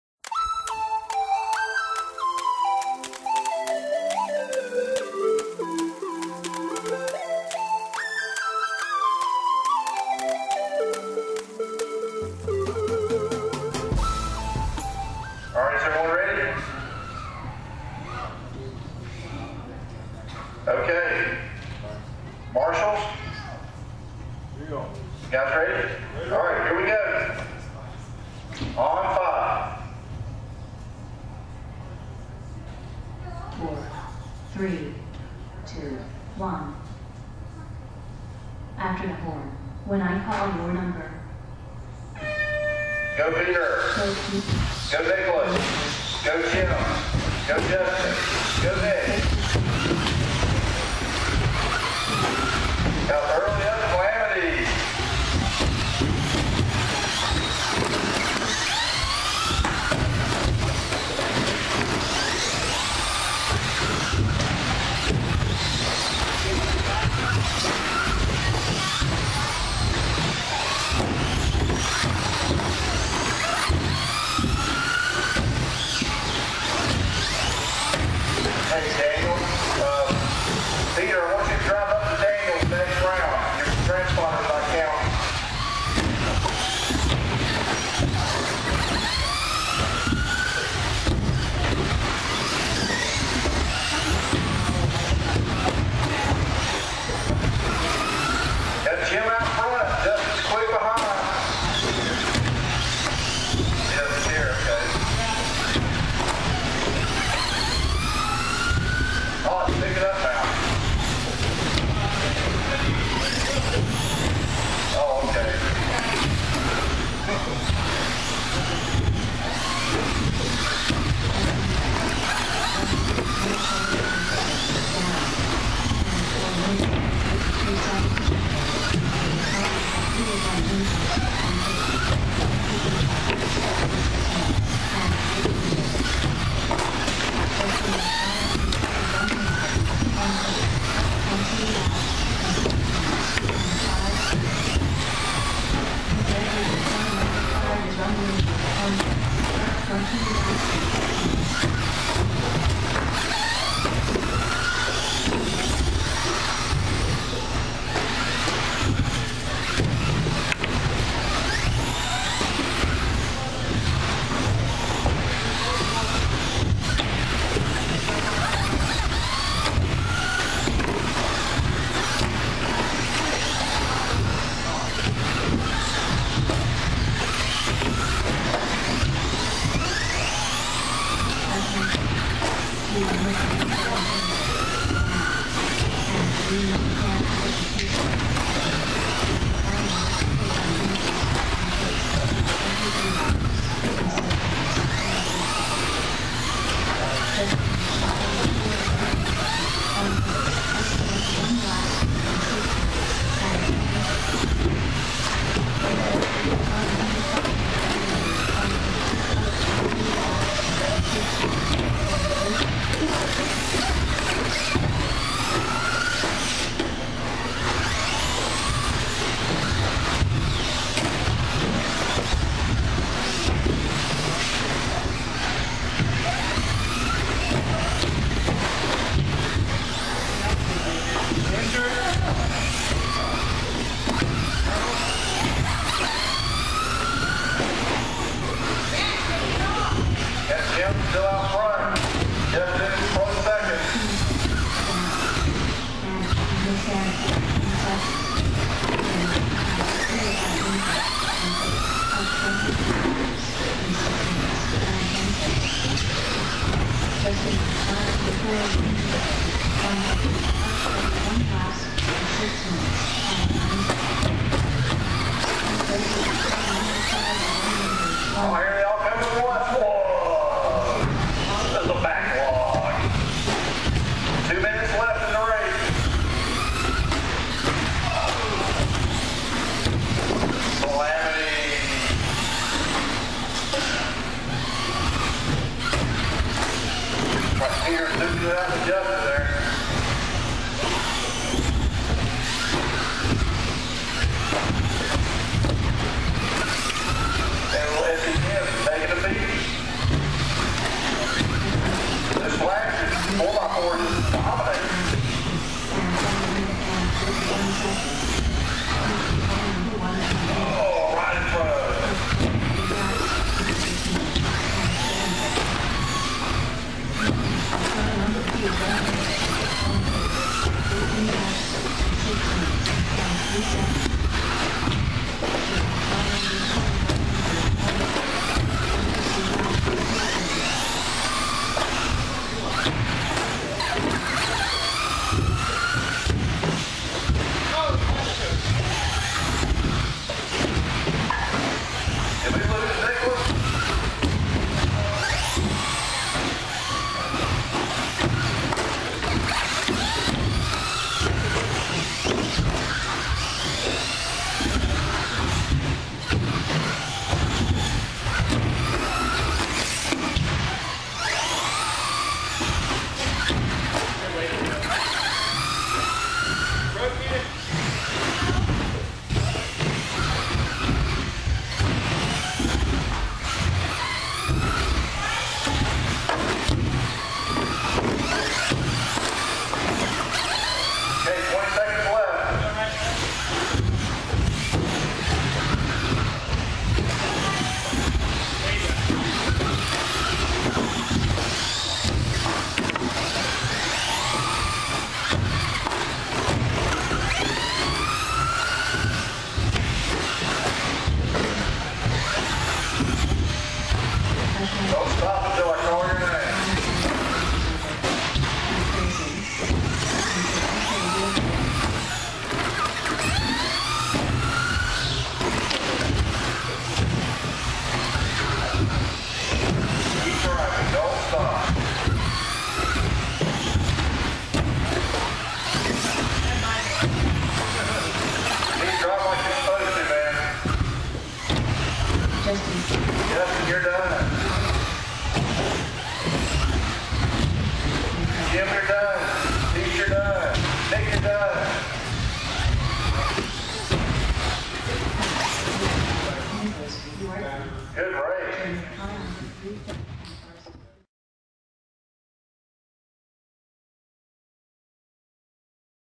HotShot RC :: SCT Qualifying Round 2 :: 2-3-12
Friday night racing and HotShot RC in Marietta, GA.

Racers:
Jim- Traxxas Slash
Justin- Traxxas Slash 4x4 Ultimate
Nick- AE SC10 Rockstar Ed.
Peter- AE SC10 Monstor Ed.
Nicholas- Hpi Blitz ESE Pro

Results:
1. Slash 4x4
2. Slash 2wd
3. SC10 Rockstar
4. SC10 Monster
5. Blitz ESE DNF